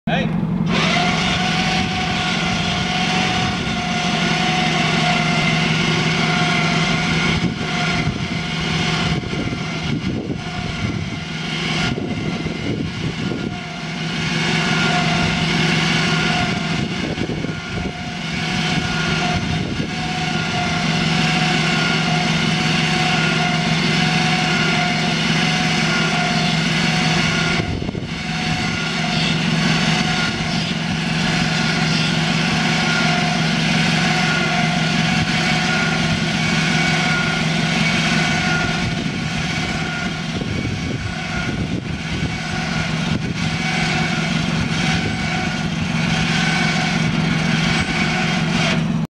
YRARC Tower project HD video 2
York Region Amateur Radio Club - tower trailer rebuild project, testing the winches.